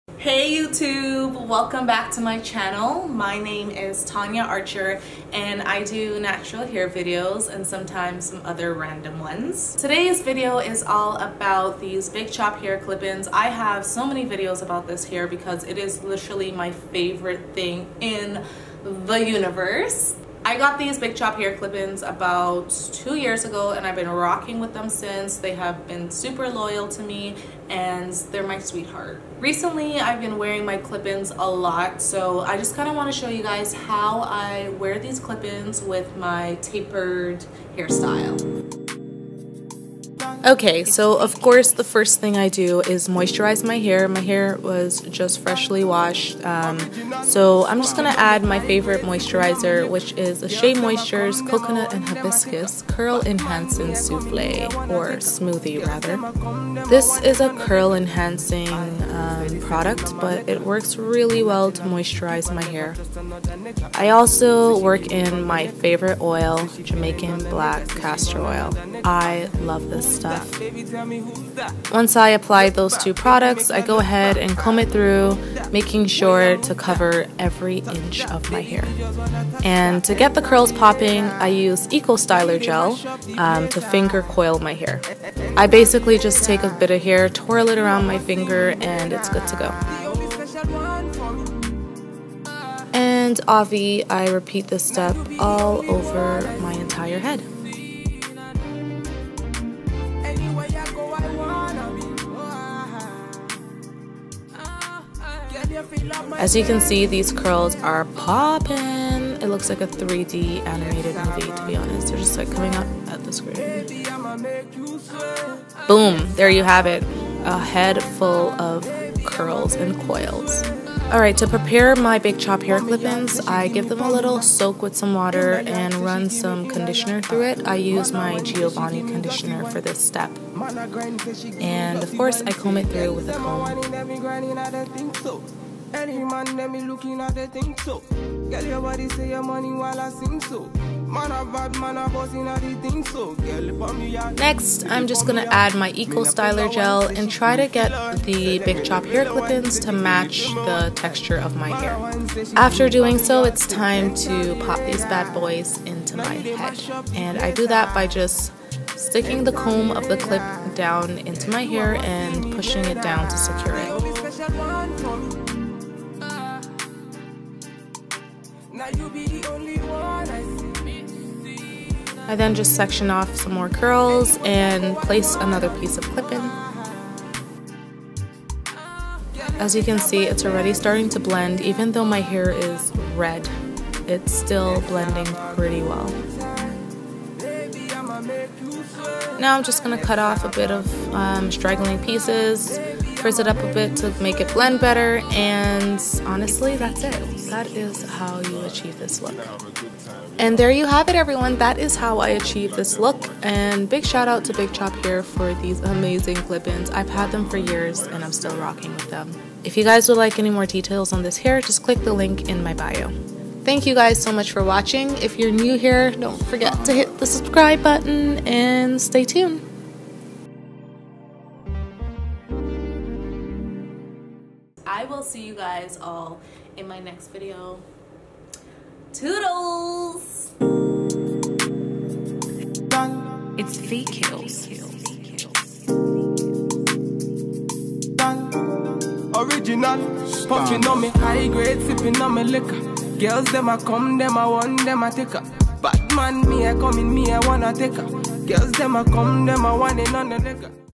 Big Chop Hair Clip Ins - Wash and Go!
I'm obviously obsessed with these clip ins! Two years later and their still going strong.

If you guys would like more details on this hair please check out my previous videos

SC: JeSuisTonya